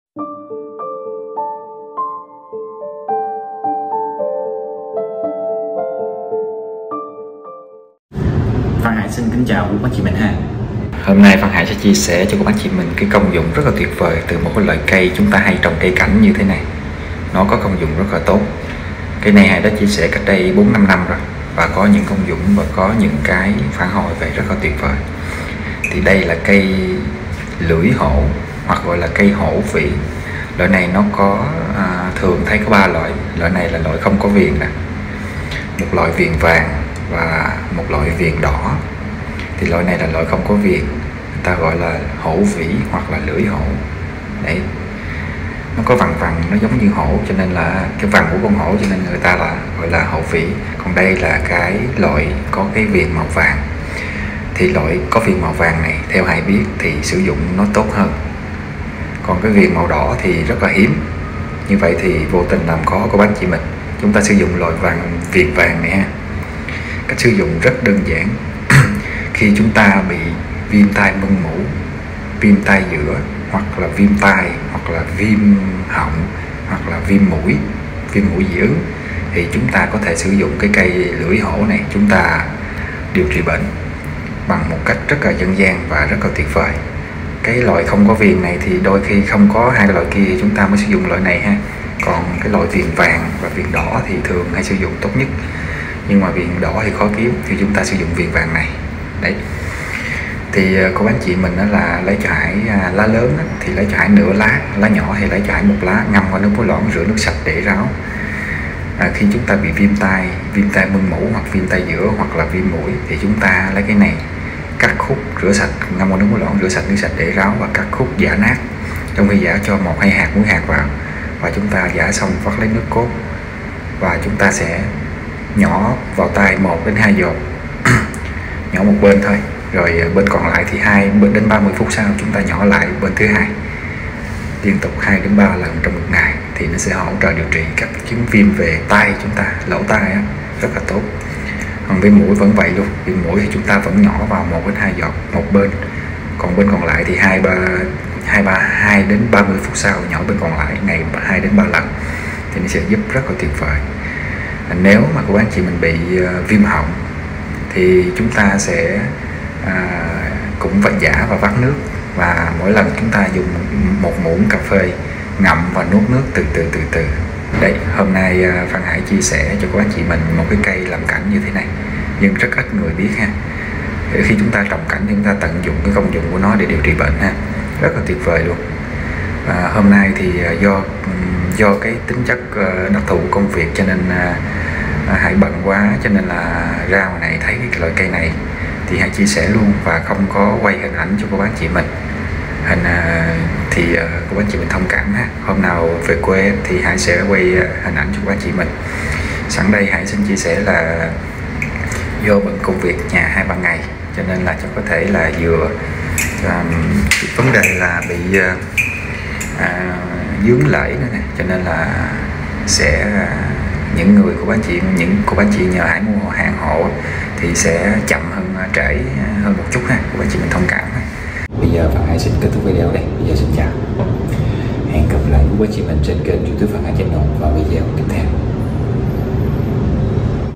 Nhiều Nhà trồng Cây này, nhưng không biết Công dụng,trị viêm tai mưng mủ, viêm họng.PHAN HẢI Channel
✅Kính chúc Quí Cô Bác Anh Chị luôn An Lạc và Hạnh Phúc
© Copyright by PHAN HẢI Channel ☞ Do not reup